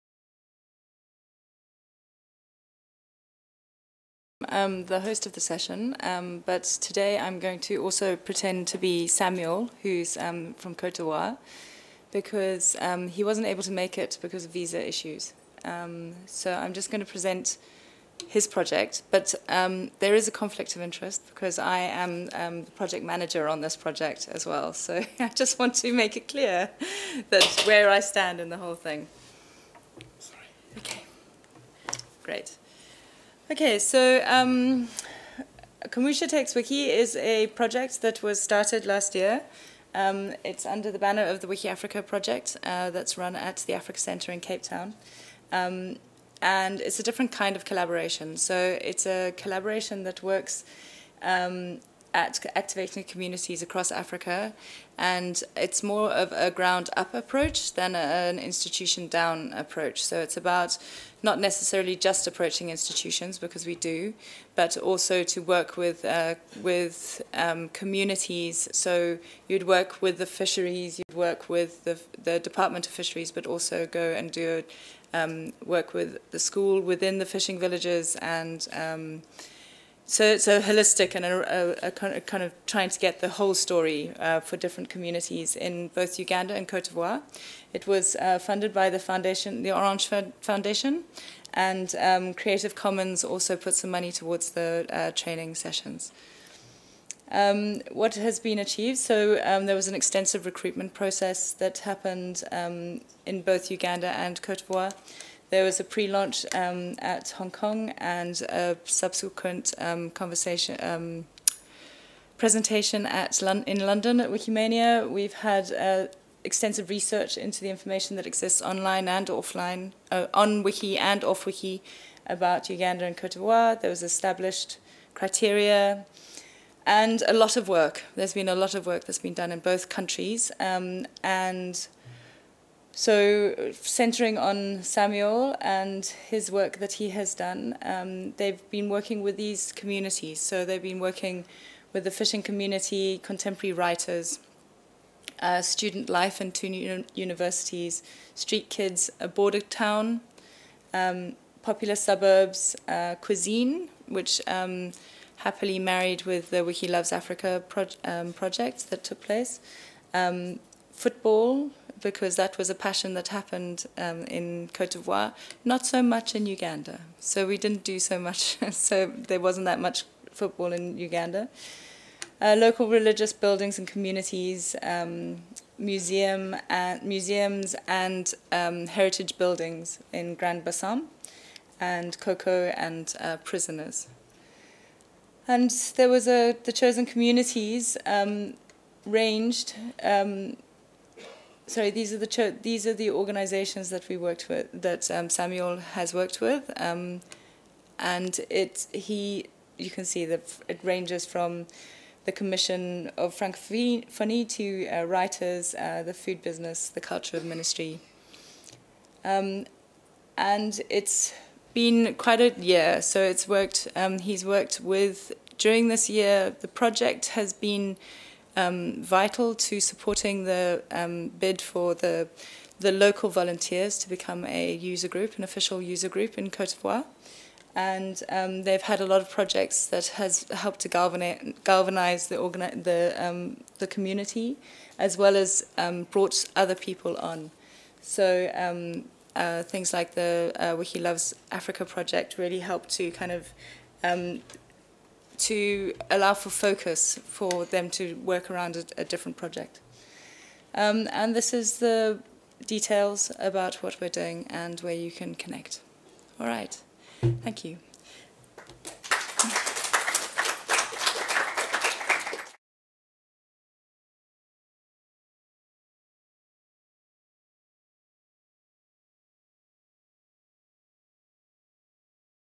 GLAM-WIKI 2015 - 11 April - Kumusha takes Wiki - Samuel Guebo
Kumusha Takes Wiki: A different kind of collaboration

Samuel Guebo, Côte d’Ivoire - represented by Isla Haddow-Flood

Kumusha Takes Wiki project aimed at creating content about some pre-identified communities or topics in Africa. Each community or topic required a specific approach. This presentation will first show the selected communities in Côte d’Ivoire and then highlight the approach followed for working with the Writers and University communities.